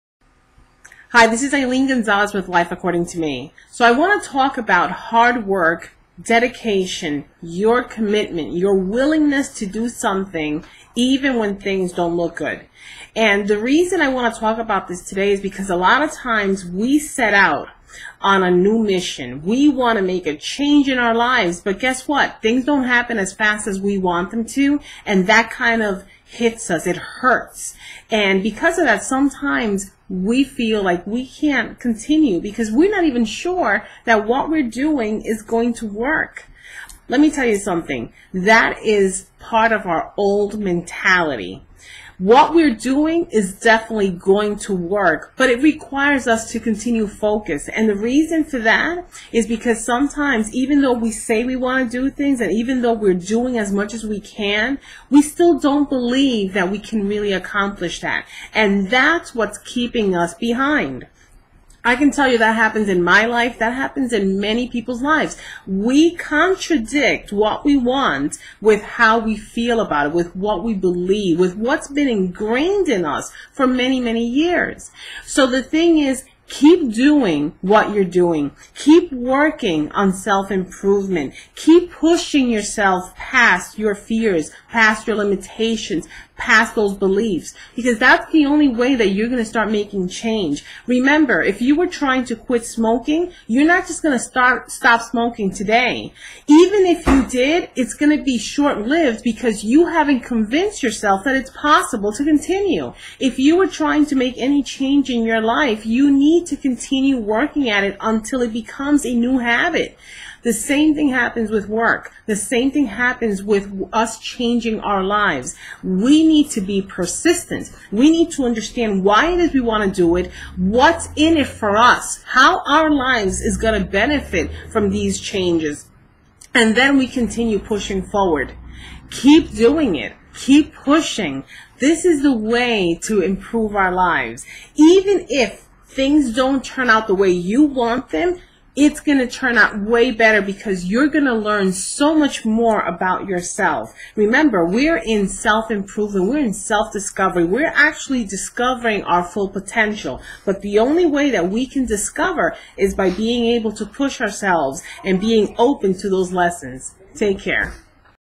Discover your full Potential
Keep pushing, Keep moving, Continue to press on.  Sometimes we feel ready to give up because we do not see things happen quick enough yet things are working, change is beginning to happen.  It may be too early to show signs yet it's happening.

Don't be too quick to stop your journey, don't be to quick to give up.
You may be closer than you think and it would be a shame to miss it just because you did not have the willingness to move a little further.